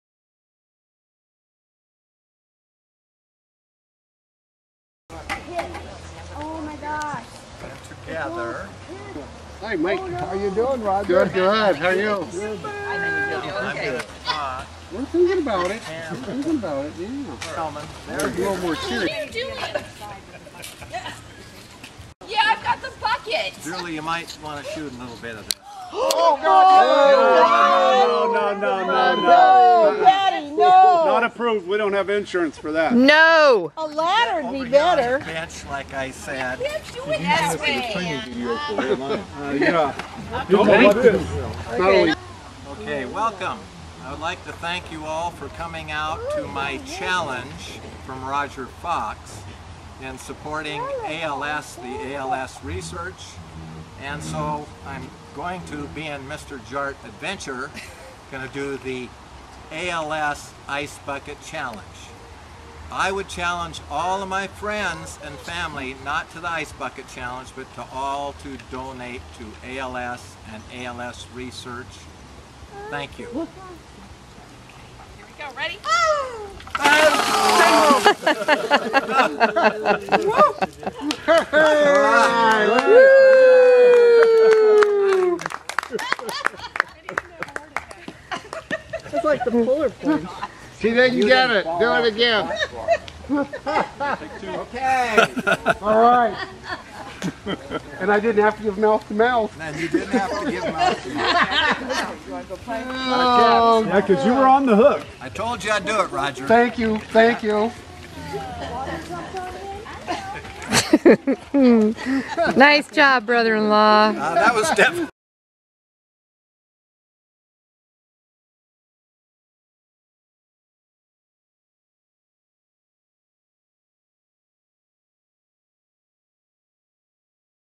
JART Adventures - ALSChallenge2014Video
This is my JART Adventure ALS Ice Bucket Challenge performed up on the Pend Oreille River with help from Julie on Video, Kim and Patti on ice Water.